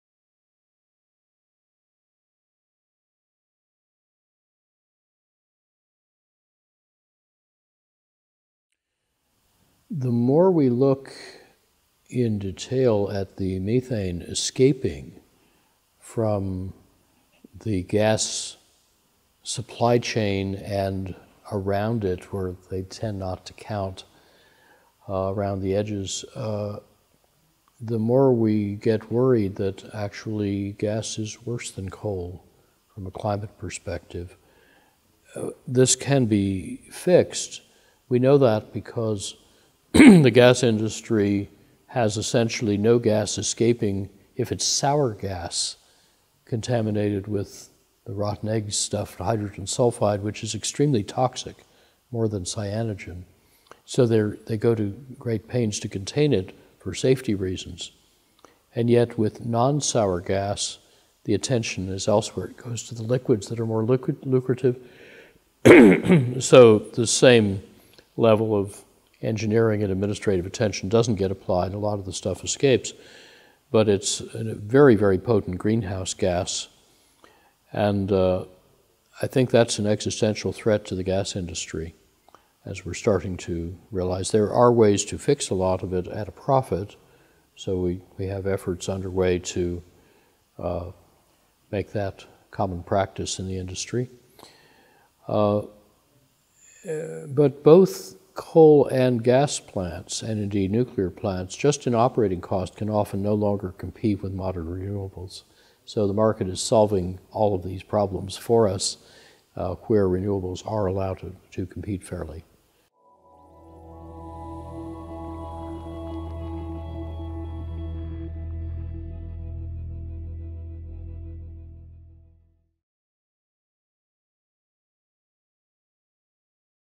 Are gas plants are good for the climate?
Carbon Brief interviewed Amory Lovins, a renowned US environmental scientist, at Chatham House in London.